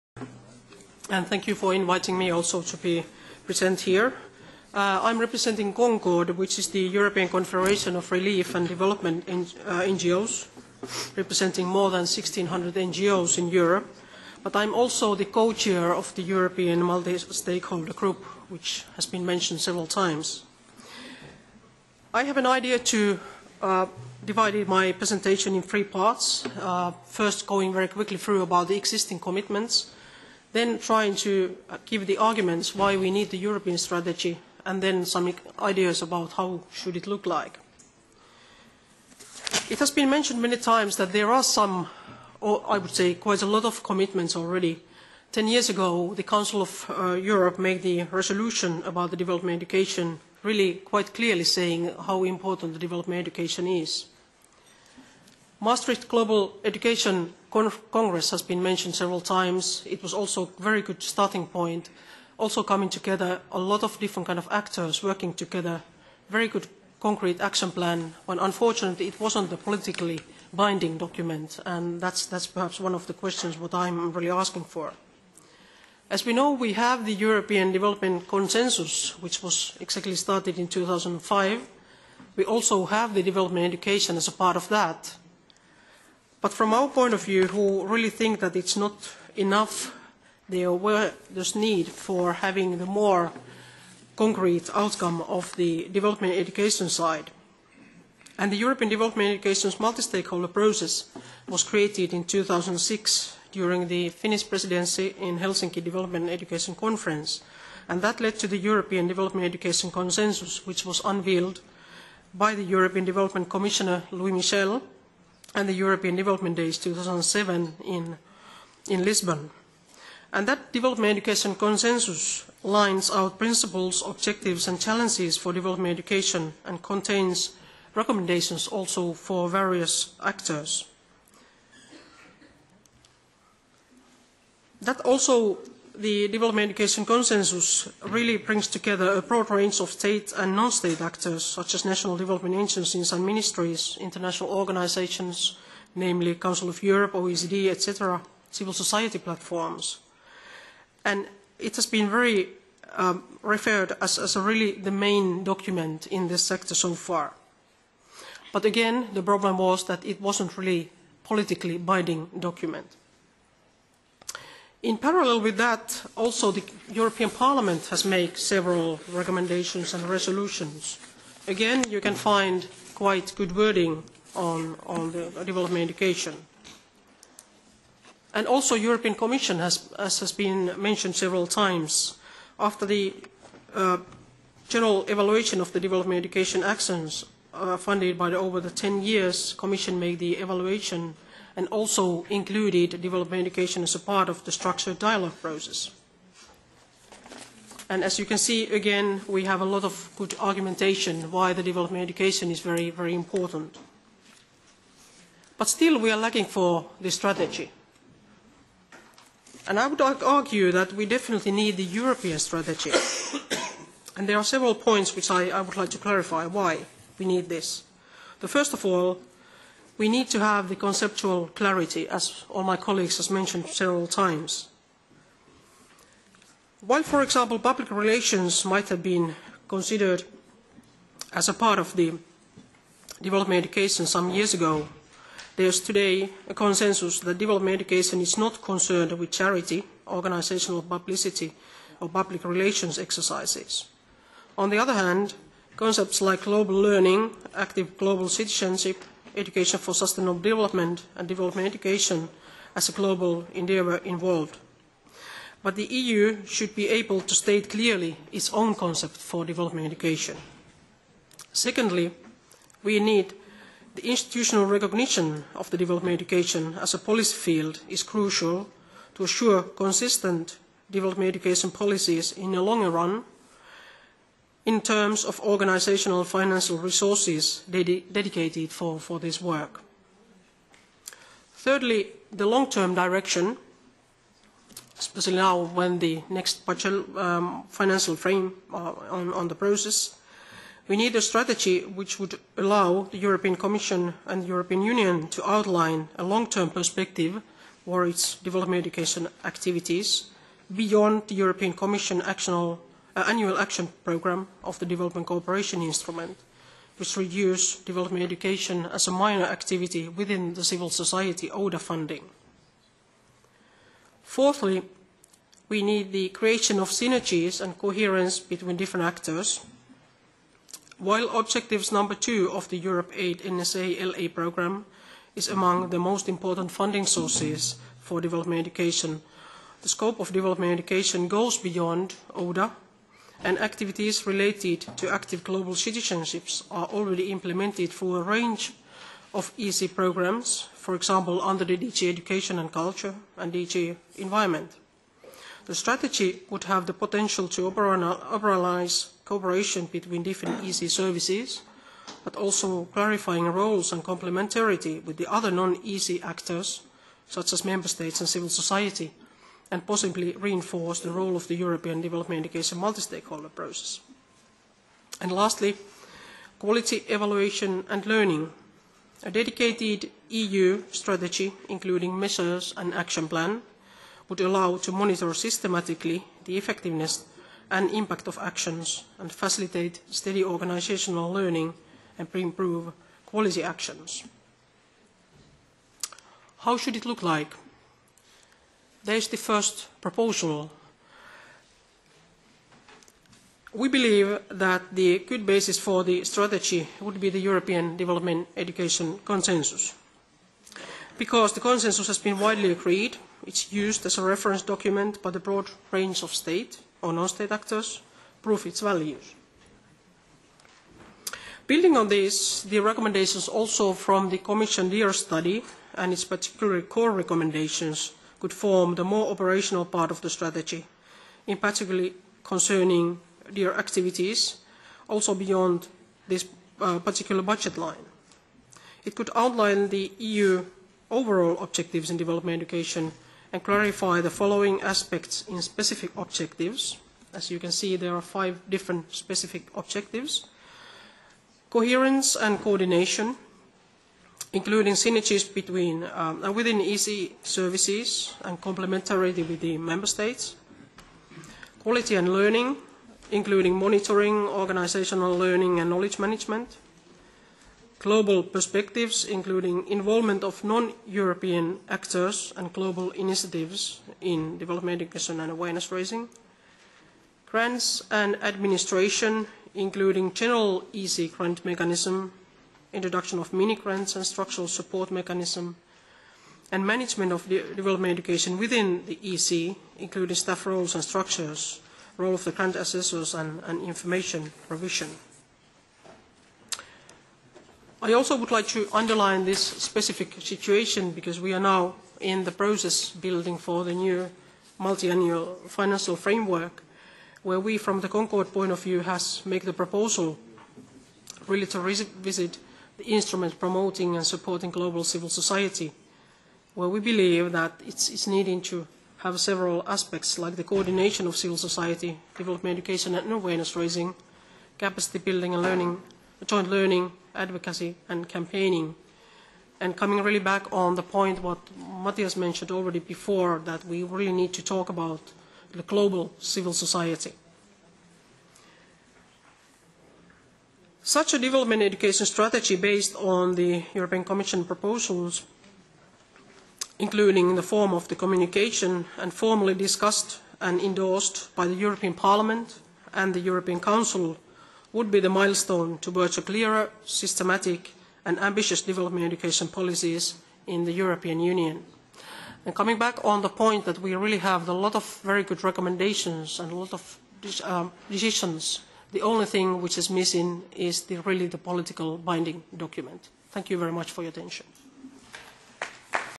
Rilli Lappalainen speech - European Parliament public hearing on Development Education
On the 30th August the first European Parliament hearing on Development Education (DE) took place.

Rilli Lappalainen proposes a possible European DEAR (Development Education Awareness Raising) strategy.